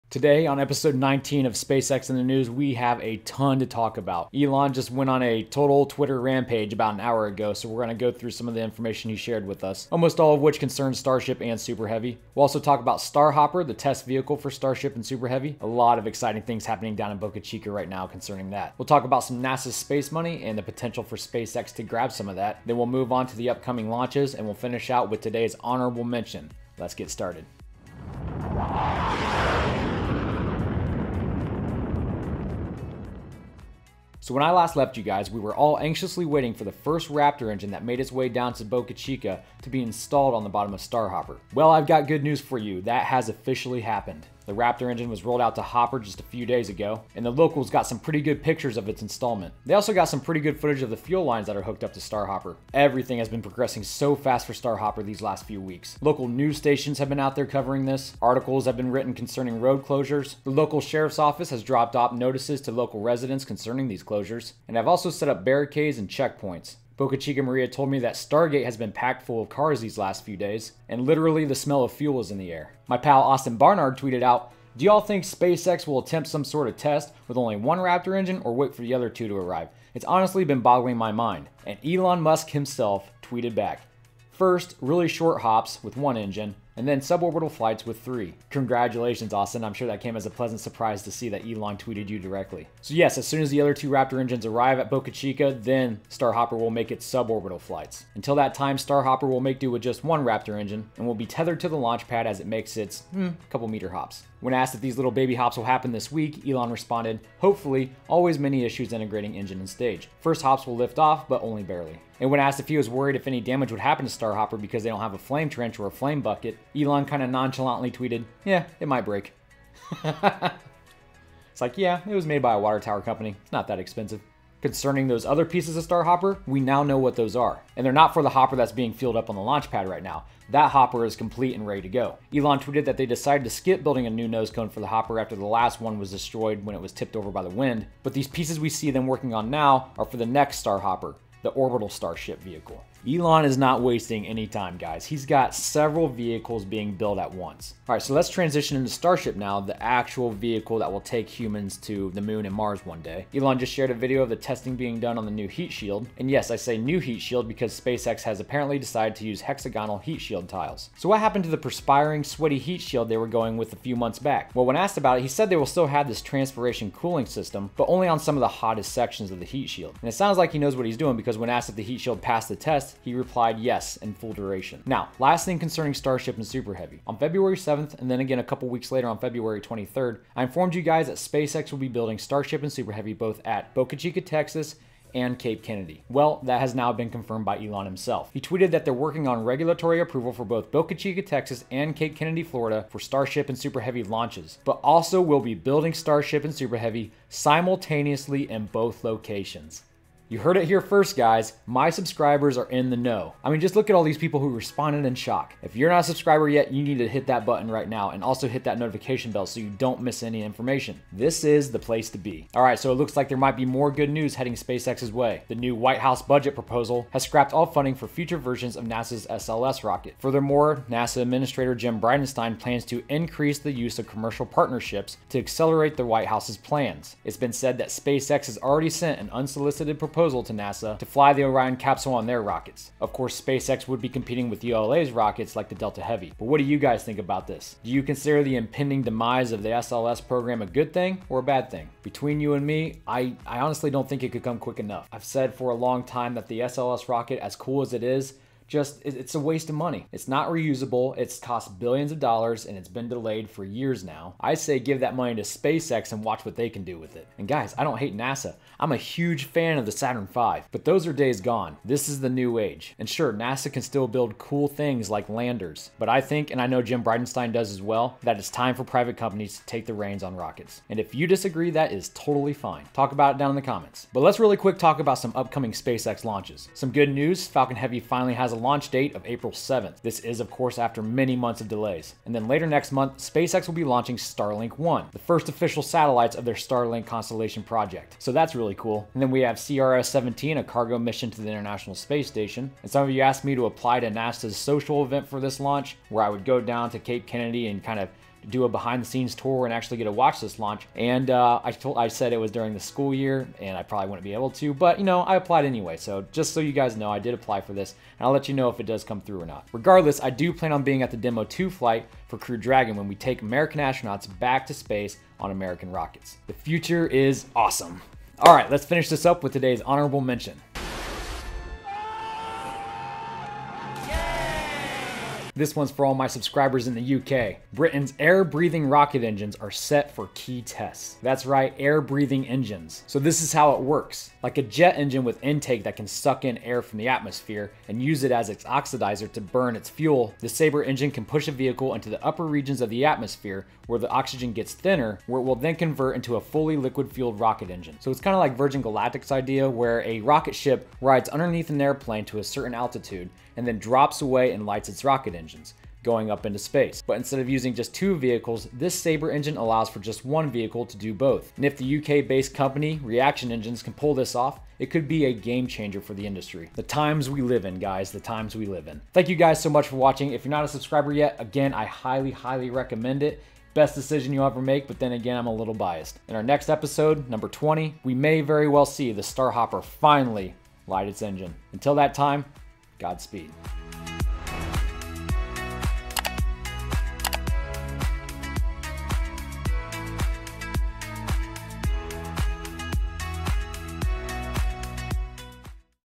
SpaceX in the News - Episode 19 (Elon Is Building 2 Starships!)
In this episode of SpaceX in the News, we take a look at Elon's tweets concerning Starship Super Heavy and Starhopper; images of raptor on the hopper down in Boca Chica; NASA's potential cutting of the SLS rocket; and future launches.

Crew Dragon Zero-G Indicator